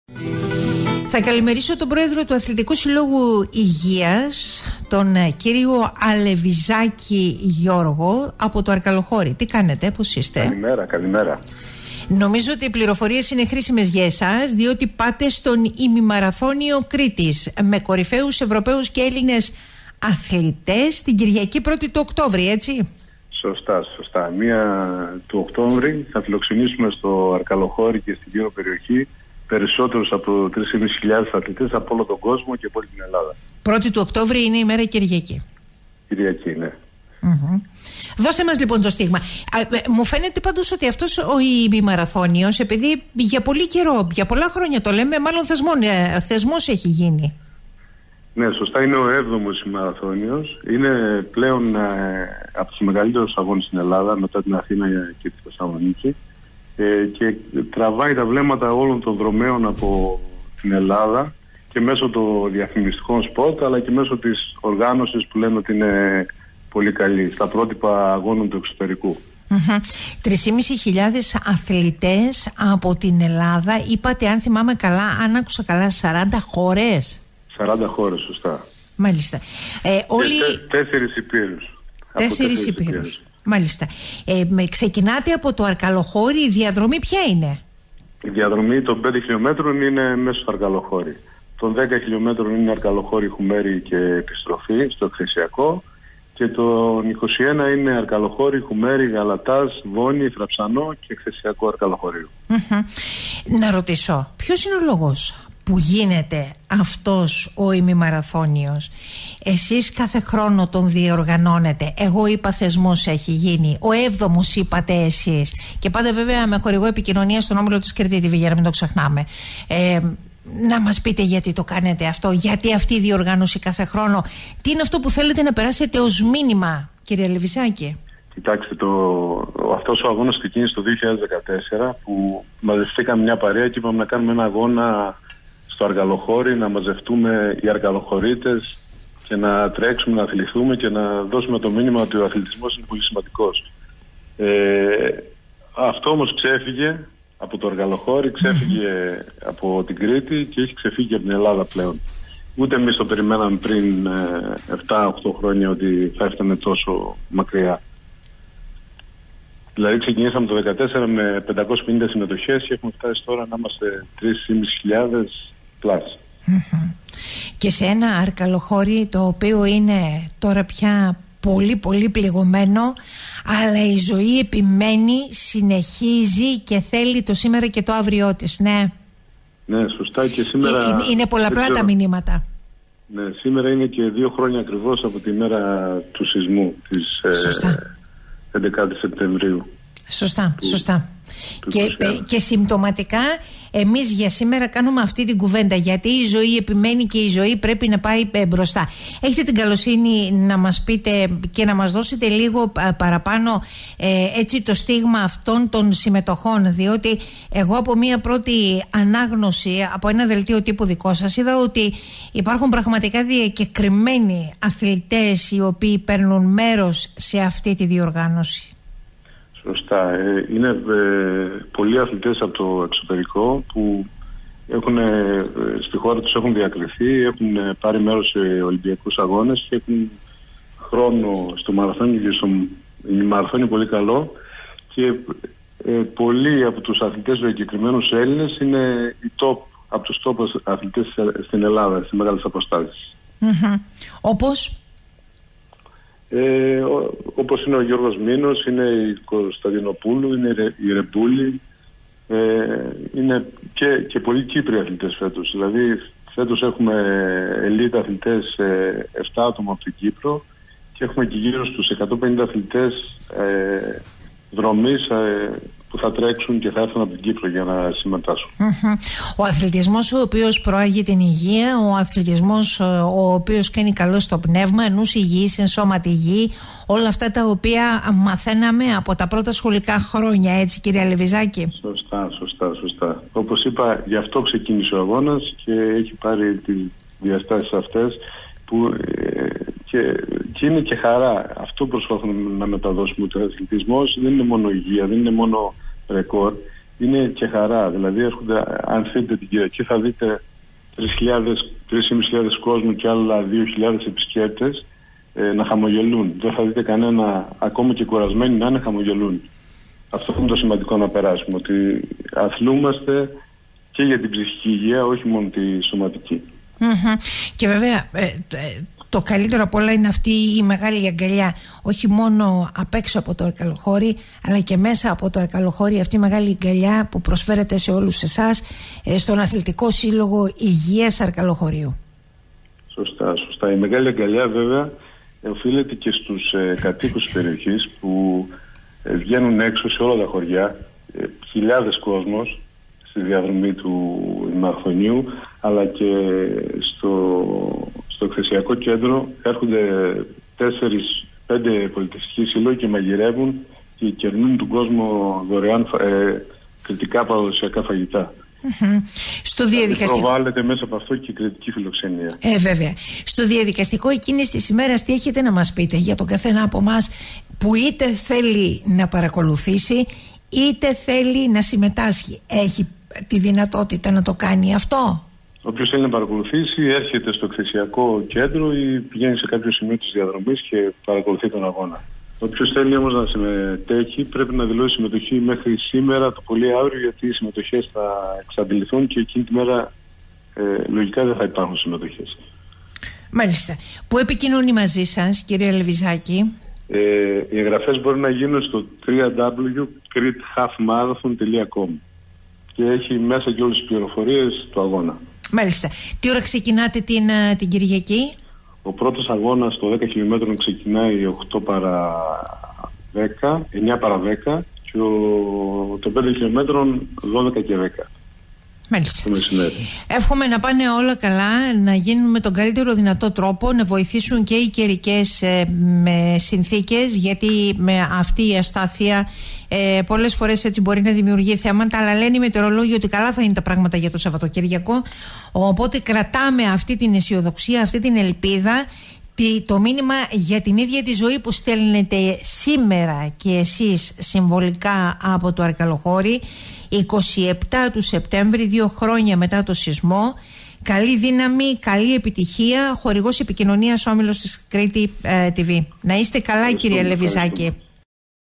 «Στο Αρκαλοχώρι ο 7ος Ημιμαραθώνιος Κρήτης»
«Ζωή κι αθλητισμός» στο Αρκαλοχώρι την Κυριακή 1η Οκτωβρίου στο πλαίσιο του 7ου Ημιμαραθώνιου Κρήτης, με τη συμμετοχή 3.500 αθλητών από την Ελλάδα κι από 40 χώρες του πλανήτη, όπως ο Γιώργος Αλεβυζάκης, Πρόεδρος Αθλητικού Συλλόγου Υγείας Αρκαλοχωρίου λέει στο Ράδιο 98.4.

Ανάμεσα τους πολλοί διακεκριμένοι αθλητές όπως  οι: «Γιάννης Ζερβάκης, Γιώργος Μίνος,  Γιώργος Τοφη,  Ιάσωνας Ιωαννίδης,  Ιωάννης Κρέκας,  Χριστόφορος Πρωτόπαπας, Γιώργος Χαράκης, Σταύρος Ζαγκανάς, Βασιλική Κωνσταντινοπούλου,  Ράνια Ρεμπούλη, Έφη Φλώρου, Αντρέας Μισαρα, Βασιλική Καλλιμογιάννη .
Χορηγός επικοινωνίας είναι ο Όμιλος ΚΡΗΤΗ TV.